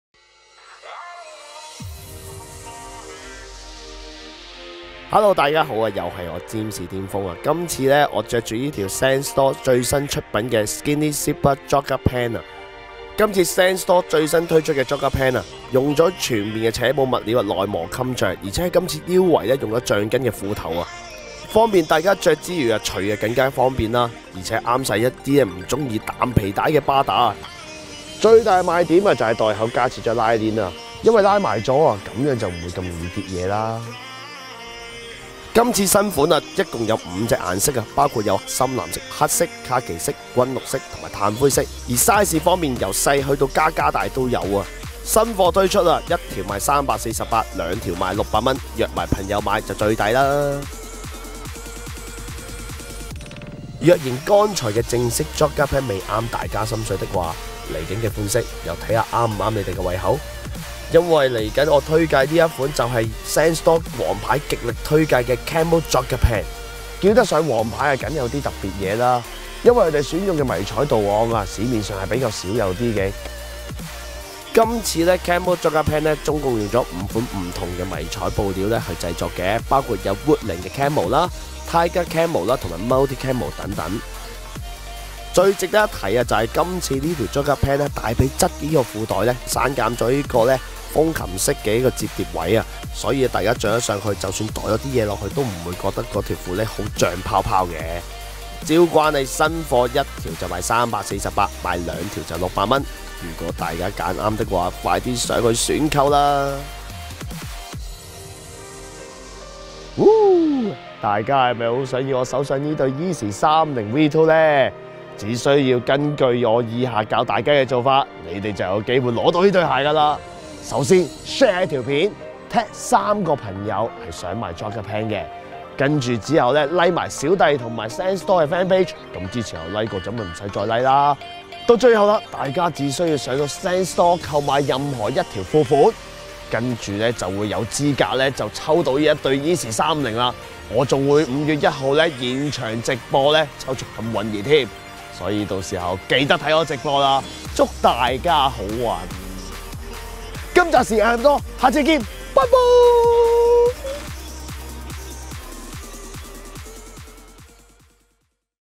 占士.甸峰 ^.^【Happy Share買褲送Yeezy！SENSE STORE​ 新款 Jogger Pants 變種登場～ 】
近年專注設計嶄新款式jogger Pants嘅SENSE STORE，最近又有兩款新上架啦！仲有終極大抽獎Tim~

而家只需要Click一下Share，兩個Like加三個Tag買埋條褲即有機會抽中Yeezy boost 350 V2(黑紅配色)。睇片你就明

而家各位係咪"很想抽吧"呢？抽獎者名額得300個，要買就快啦！咁簡單嘅事就係人都明白啦